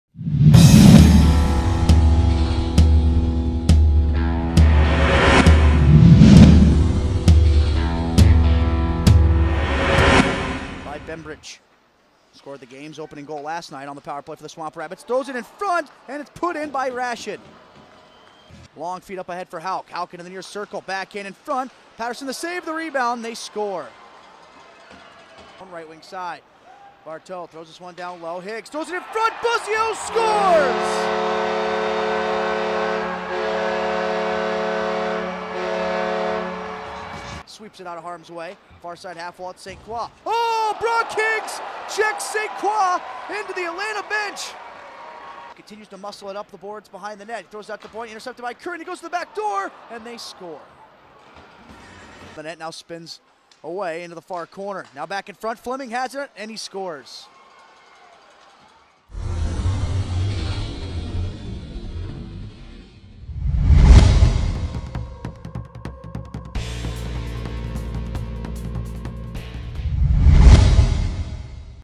Atlanta Gladiators Highlights: 3/6 vs. GRN
The Gladiators faced off against the Swamp Rabbits Sunday afternoon for the second time in as many times, and much like Saturday’s result - Greenville came away with the win and the crucial two points. Justin Buzzeo contributed the Glads lone goal.